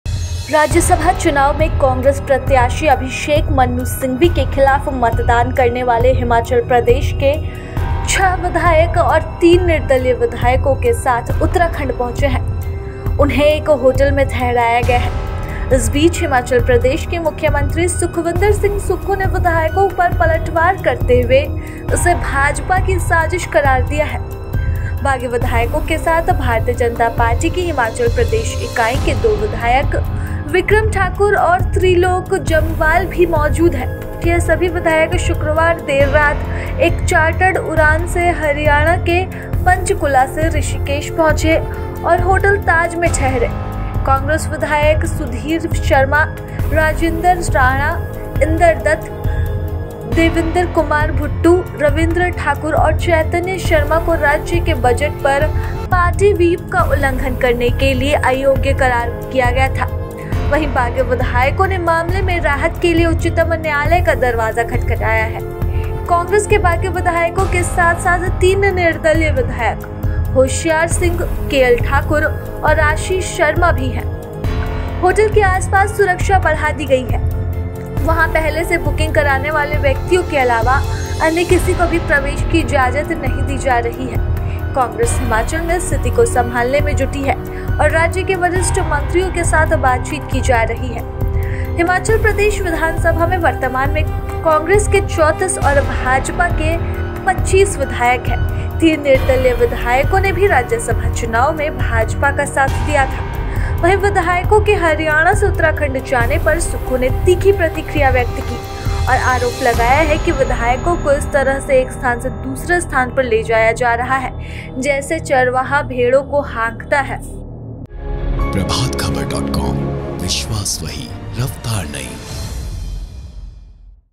Himachal Pradesh Politics: विधायकों को उत्तराखंड ले जाने पर भड़के सीएम सुक्खू
राज्यसभा चुनाव में कांग्रेस प्रत्याशी अभिषेक मनु सिंघवी के खिलाफ मतदान करने वाले हिमाचल प्रदेश में पार्टी के छह विधायक तीन निर्दलीय विधायकों के साथ उत्तराखंड पहुंचे और उन्हें एक होटल में ठहराया गया है। इस बीच, हिमाचल प्रदेश के मुख्यमंत्री सुखविंदर सिंह सुक्खू ने विधायकों पर पलटवार करते हुए इसे भाजपा की ‘‘साजिश’’ करार दिया। बागी विधायकों के साथ भारतीय जनता पार्टी (भाजपा) की हिमाचल प्रदेश इकाई के दो विधायक विक्रम ठाकुर और त्रिलोक जमवाल भी मौजूद हैं। ये सभी विधायक शुक्रवार देर रात एक चार्टर्ड उड़ान से हरियाणा के पंचकुला से ऋषिकेश पहुंचे और होटल ताज में ठहरे। कांग्रेस विधायक सुधीर शर्मा, राजिंदर राणा, इंदर दत्त लखनपाल, देविंदर कुमार भुट्टू, रवींद्र ठाकुर और चैतन्य शर्मा को राज्य के बजट पर पार्टी व्हिप का उल्लंघन करने के लिए अयोग्य घोषित कर दिया गया है।